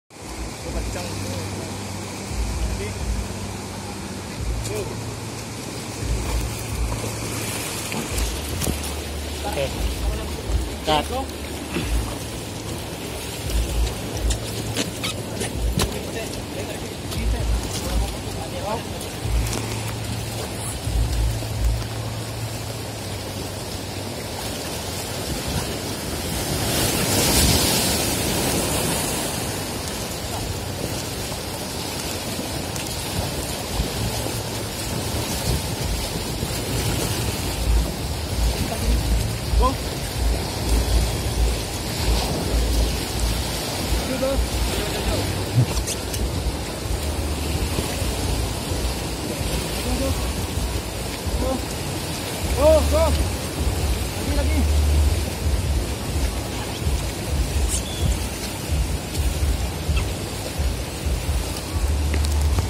Kejuaraan Arung Jeram Terbuka se Sumatera Barat Halaban
Inilah sungai lokasi diselenggarakan nya kejuaraan arung jeram terbuka R 4 di kecamatan lareh sago halaban, kejuaraan ini diikuti sebanyak 30 tim dari berbagai daerah. Diselenggarakan mulai 10-11 Desember 2019